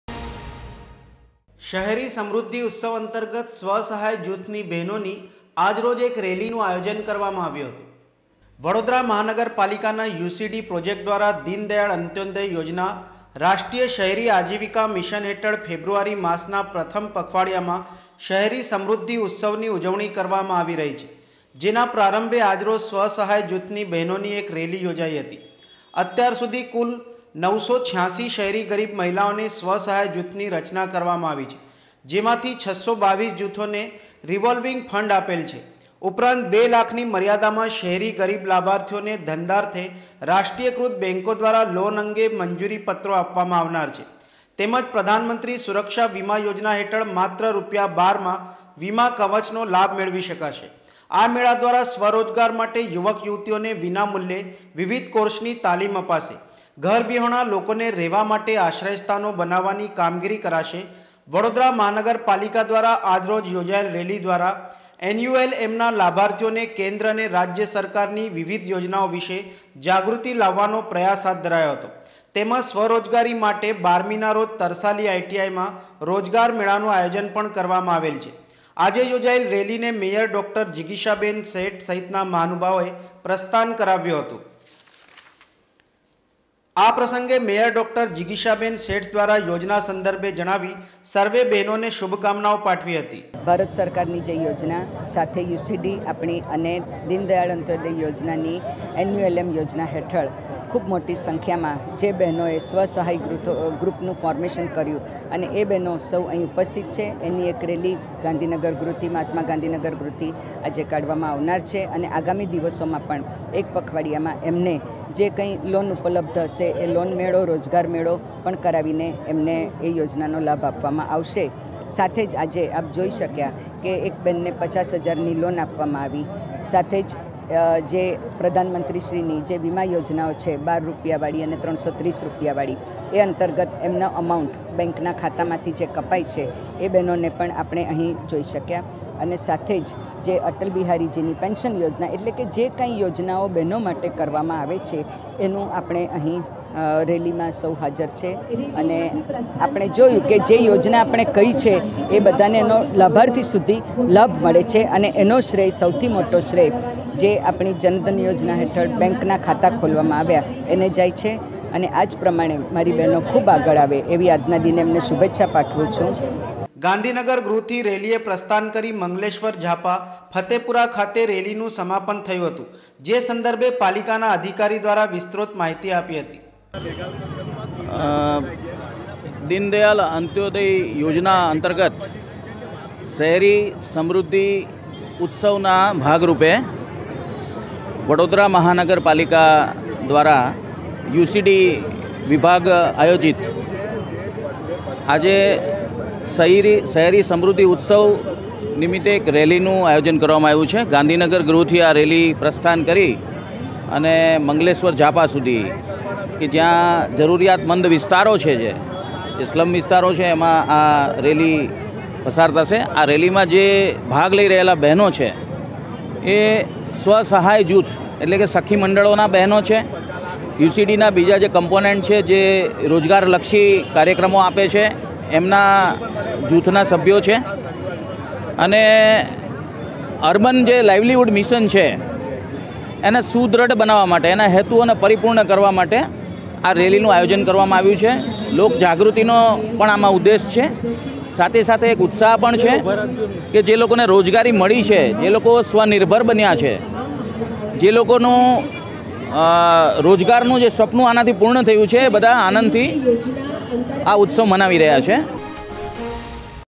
LOKMAT NEWS VADODARA 01-02-19
રાષ્ટ્રીય શહેરી આજીવિકા મિશન અંતર્ગત શહેરી સમૃદ્ધિ ઉત્સવ ની ઉજવણી કરાઈ